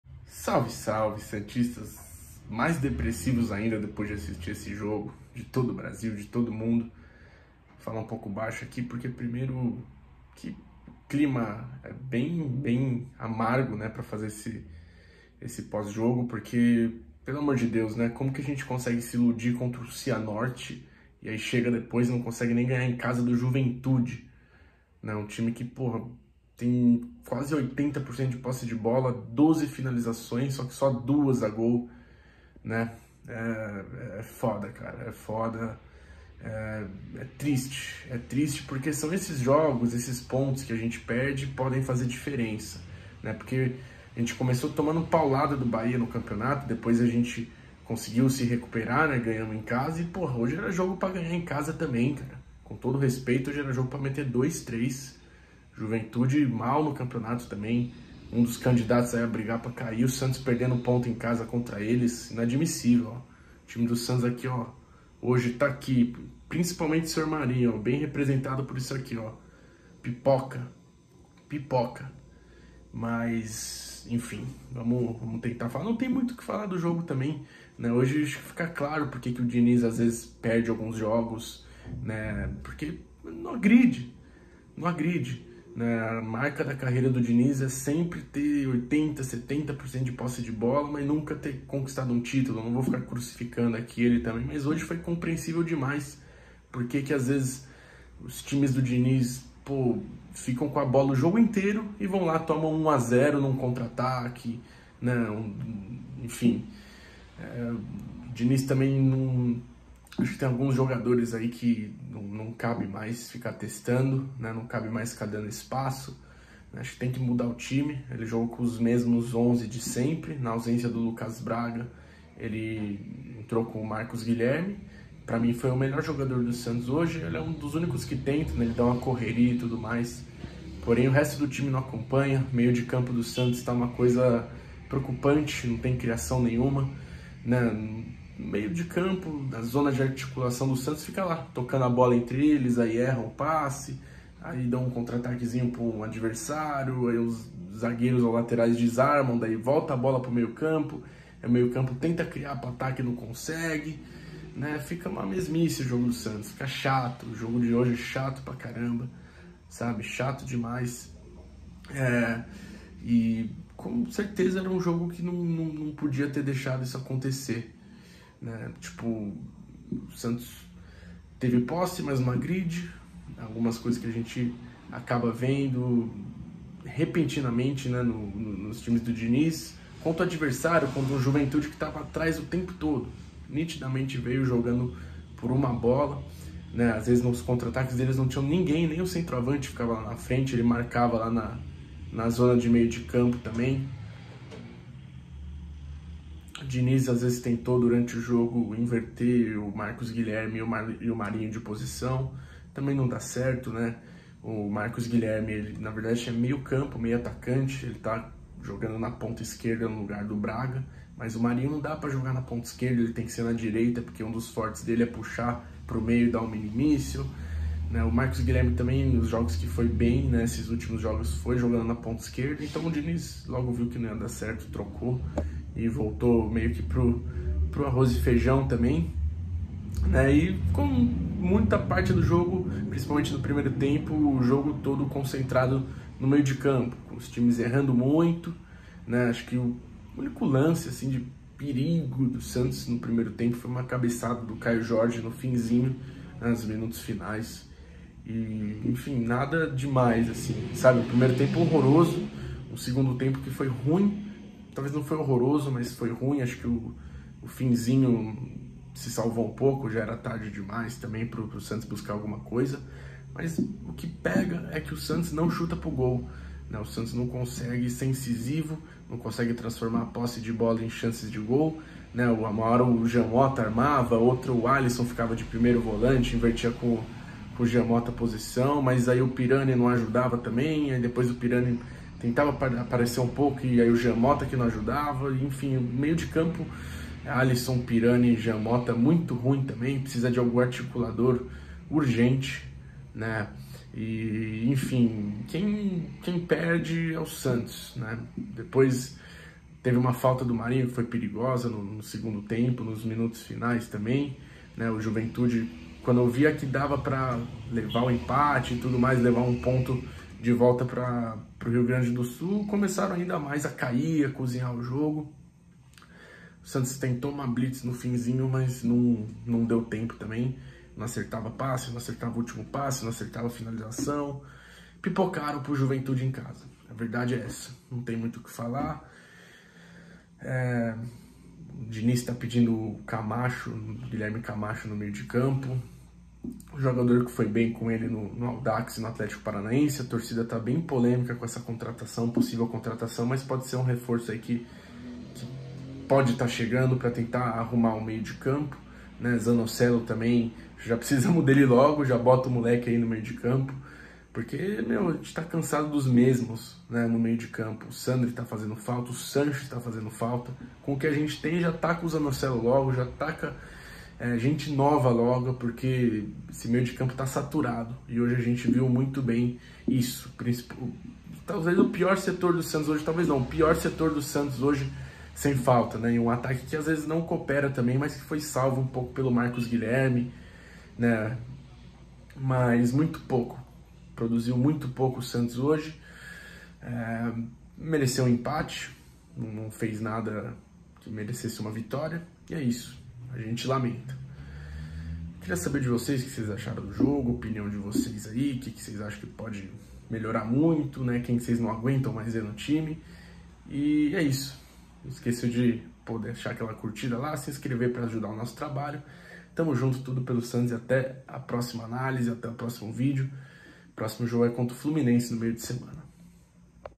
SÃO ESSES PONTOS QUE FAZEM FALTA NA FRENTE | Pós-jogo de Santos 0x0 Juventude (Brasileirão 2021)
Empatamos sem gols com o Juventude em casa em um jogo fraquíssimo, chato, onde o Santos teve muita posse de bola mas não soube criar e nem ser efetivo.